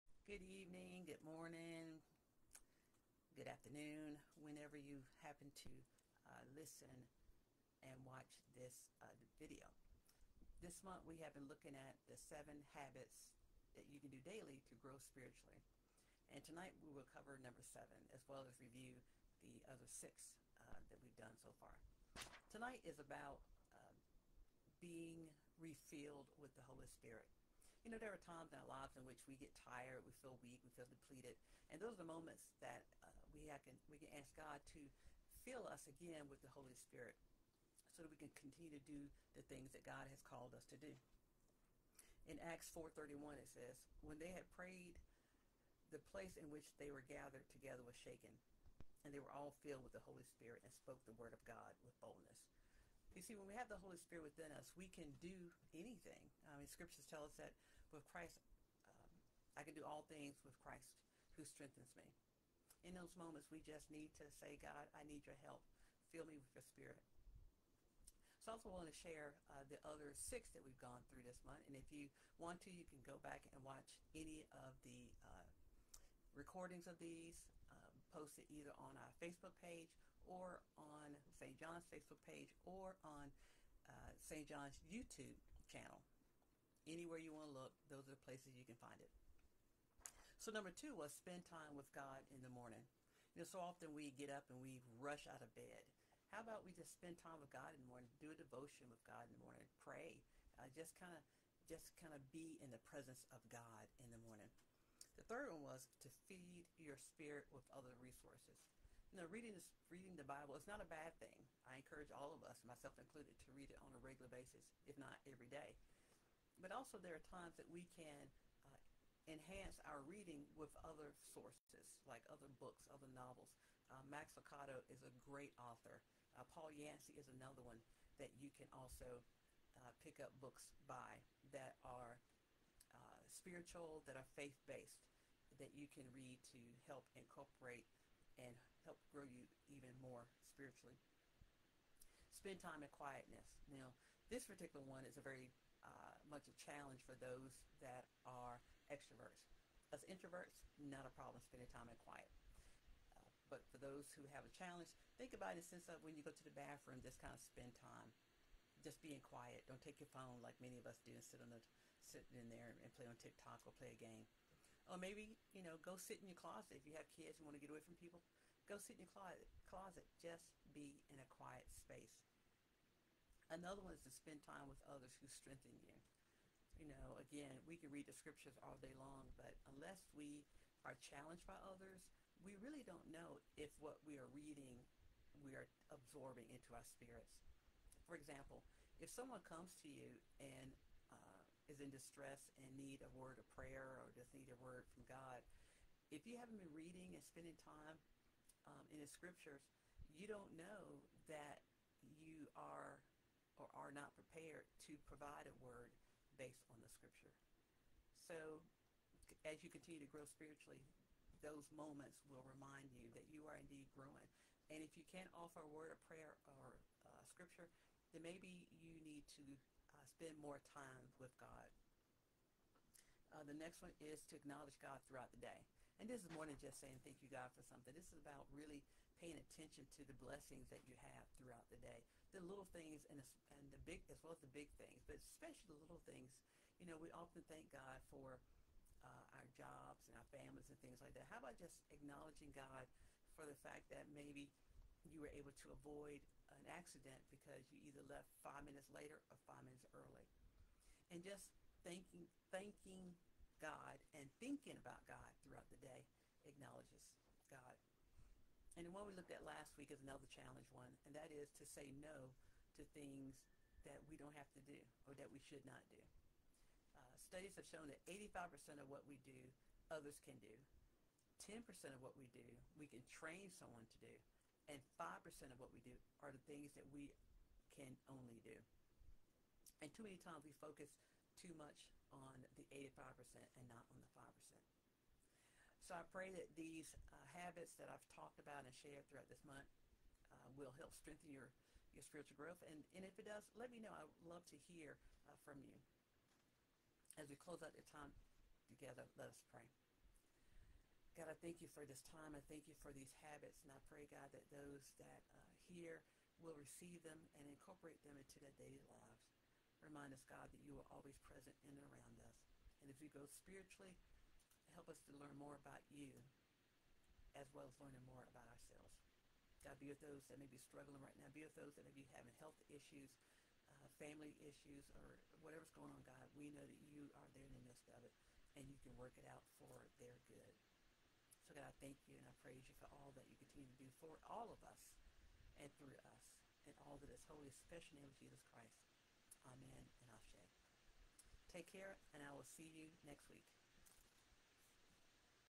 PM Midweek Manna 9/27 #7 Habit
#7 - Be Refilled by the Holy Spirit
There are times we are in need of the Holy Spirit to strengthen us